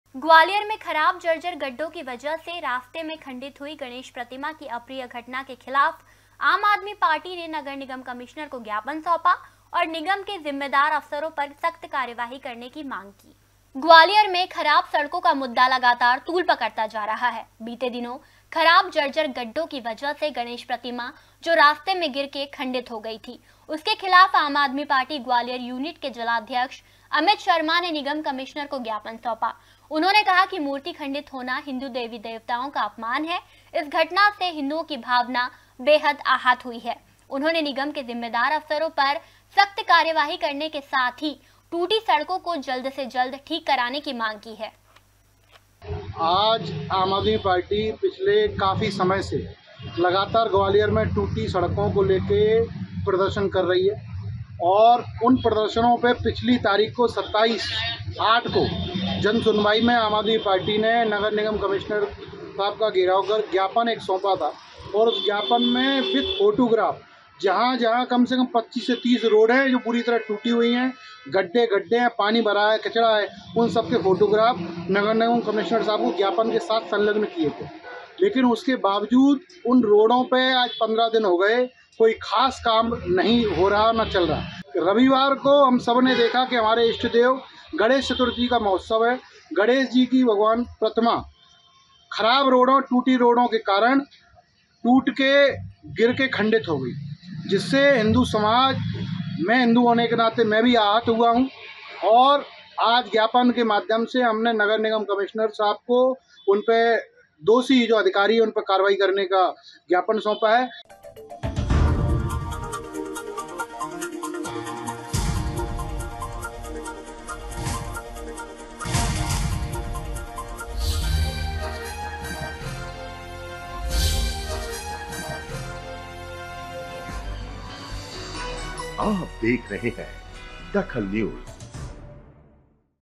DAKHAL NEWS खराब सड़कों के कारण गणेश प्रतिमा हुई थी खंडित
खराब सड़कों के कारण गणेश प्रतिमा हुई थी खंडित
जिम्मेदारों पर कार्यवाही को लेकर आप ने दिया ज्ञापन